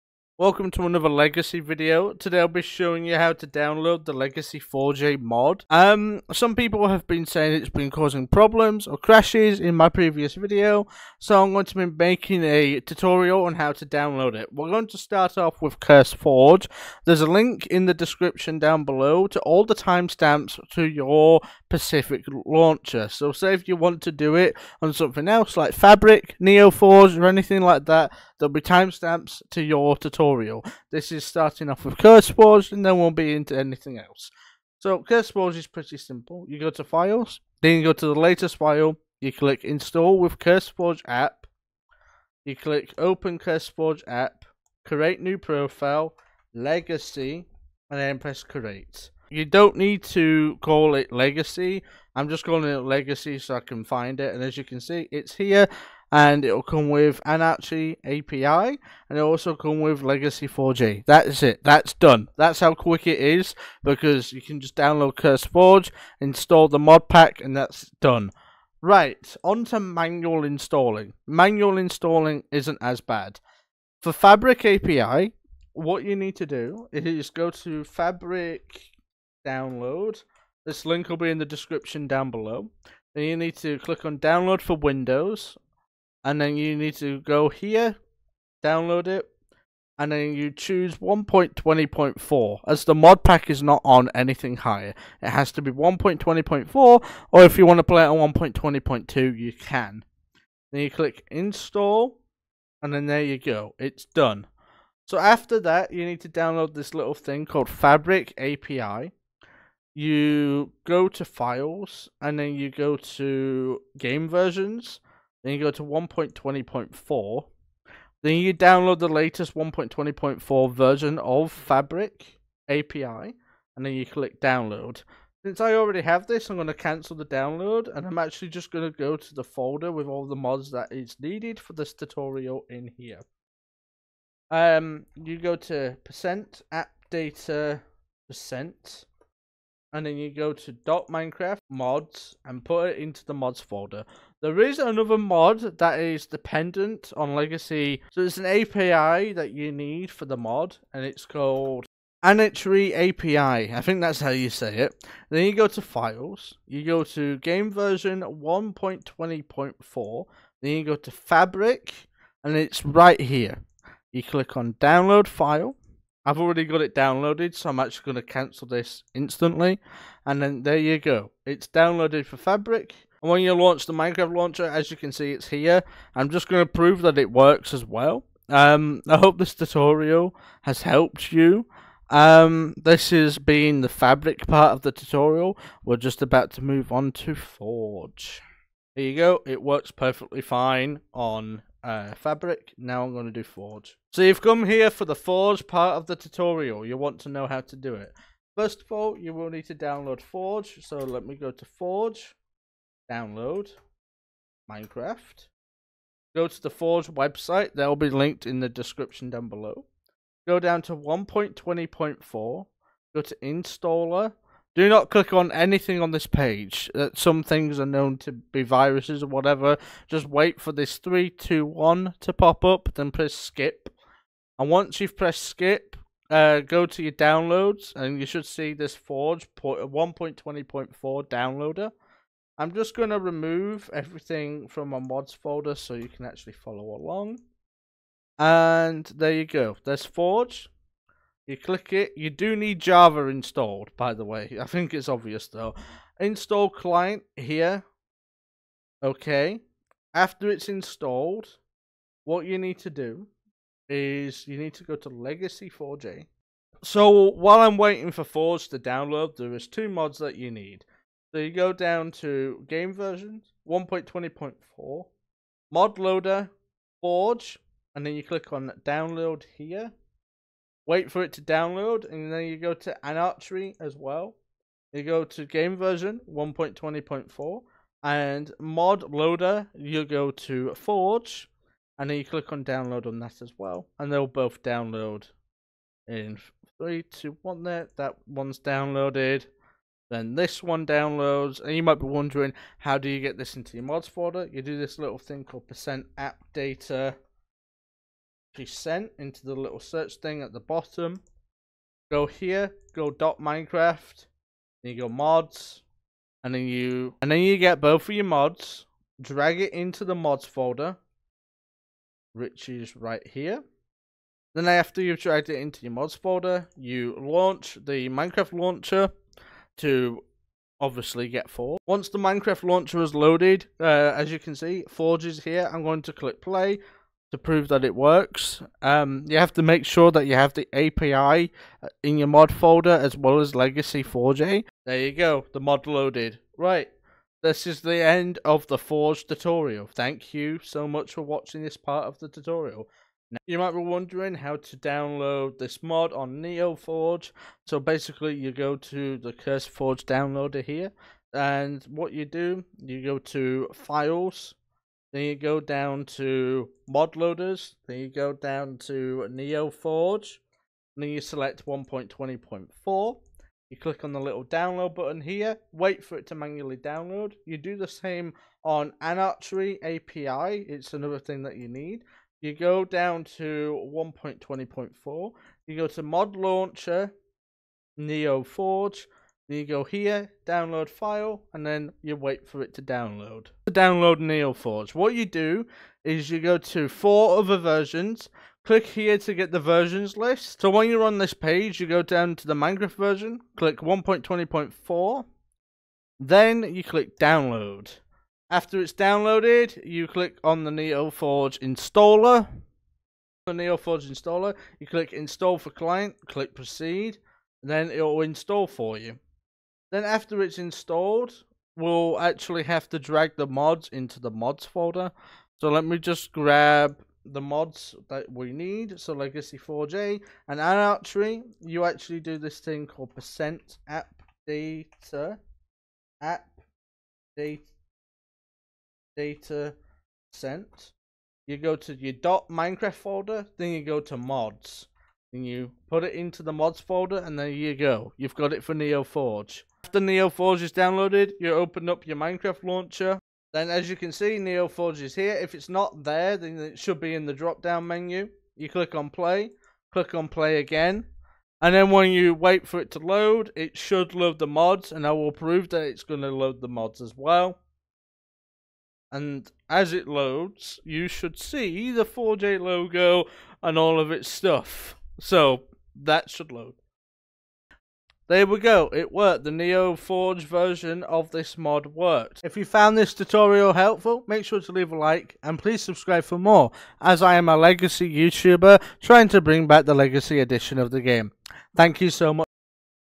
How To Download The Legacy4J Modpack (Check Description)
Downloads
Fabric

Mod downloads
Legacy4J

Timestamps

0:00 : Curseforge Tutorial
1:13 : Fabric Tutorial
3:14 : Forge Tutorial
5:54 : Neo Forge Tutorial
7:47 : Outro

In Todays Video, I am going to be doing